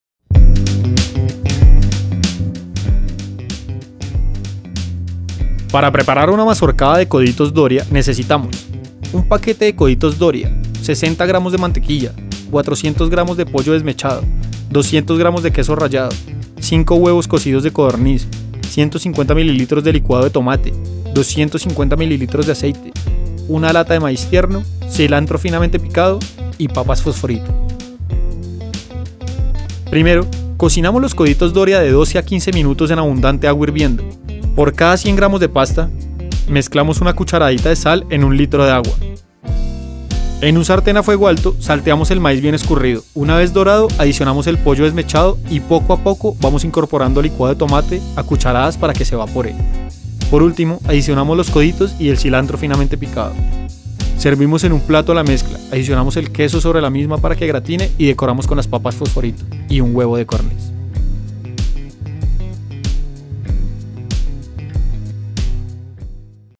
mazorcada de coditos doria
Ingredientes:

- 60g de mantequilla
- 400g de pollo desmechado
- 200g de queso rallado
- 5 huevos cocidos de codorniz
- 150ml de licuado de Tomate
- 250ml de aceite
- 1 lata de maíz tierno
- Cilantro finamente picado
- Fosforitos (papas)

Preparación:

1. Cocinamos los Coditos Doria de 12 a 15 minutos en abundante agua hirviendo.

2. Por cada 100 gr de pasta mezclamos una cucharadita de sal en 1 litro de agua. Adicionamos la mantequilla.

3. En un sartén a fuego alto salteamos el maíz bien escurrido, una vez dorado adicionamos el pollo desmechado y poco a poco vamos incorporando licuado de tomate a cucharadas para que se evapore.

4. Por último adicionamos los coditos y el cilantro finamente picado.

5. Servimos en un plato la mezcla, adicionamos el queso sobre la misma para que gratine y decoramos con los Fosforitos y un huevo de codorniz.